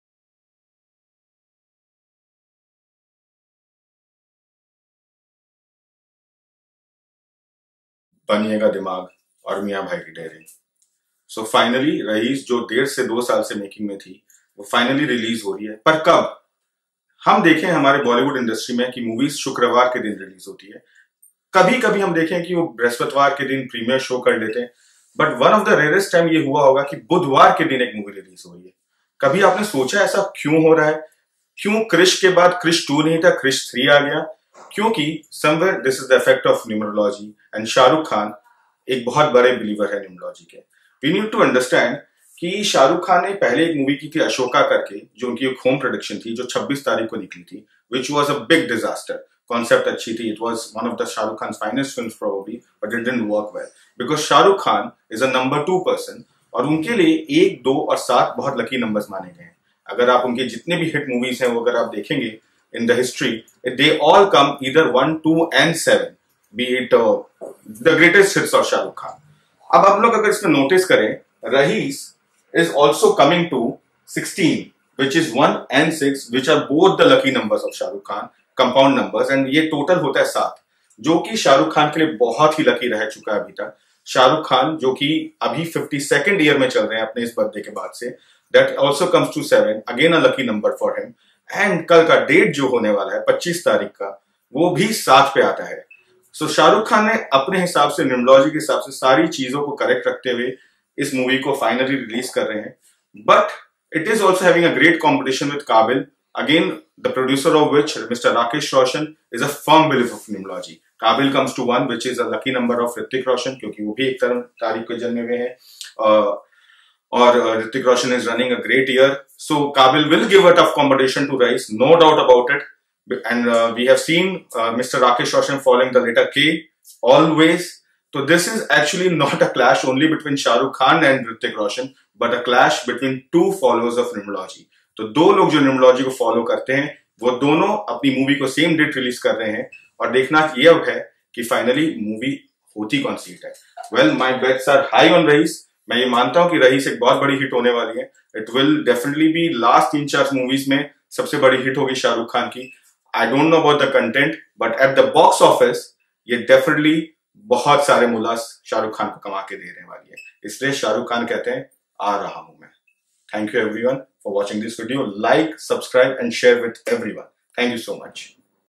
Raees Movie| Numerology- Whats in store for Raees movie
Raees Movie is finally releasing after a long delay. SRK who is a believer in Numerology and Astrology ( KKR jersey changed from black to purple and kkr lifts trophy the maximum time. Wearing white at stadium, 555 car number, etc.) has preponed the date of the movie from 26th to 25th, which is a wednesday, a rare day to release a bollywood movie. Most of his biggest box-office gainers are named 1,2,7 or are released on these dates.

The good part is the other movie which is releasing on the same day is KAABIL, coming to number 1, lucky number of Hrithik Roshan. His father is a firm believer of the science, most of his movies starts with K (remember Krish and Krish 3, no Krish 2) and 7 is a lucky number of Hrithik. Thus, this movie will cement Hrithik's position is an actor even further and shall win him accolades.

The sad part is, they might each others profits and might not get their dues.

Overall, Raees is set to become the biggest box-office gainer of SRK in recent times and for sure give him a much needed hit.

Though, I can't guarantee you the content of the movie.

A lotus to you,
a buddha to be.

MasterRishav